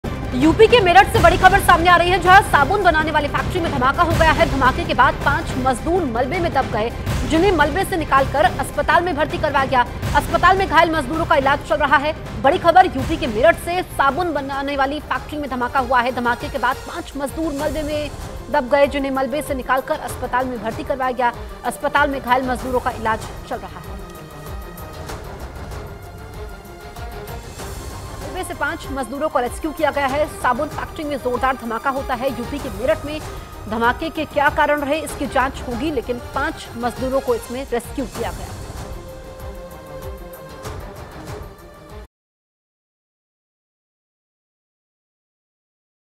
Breaking News: UP के Meerut में साबुन Factory में धमाका, 5 मज़दूर अस्पताल में भर्ती | news18
Breaking News: There was an explosion in a soap manufacturing factory in Meerut, UP. After the blast, five workers were buried under the debris and were rescued from the debris and admitted to the hospital. The injured workers are undergoing treatment in the hospital.

Breaking News: यूपी के मेरठ में  साबुन बनाने वाली फ़ैक्टरी में धमाका हो गया. धमाके के बाद पांच मज़दूर मलबे में दब गए जिन्हें मलबे से निकाल कर अस्पताल में भर्ती कराया गया. अस्पताल में घायल मज़दूरों का इलाज चल रहा है.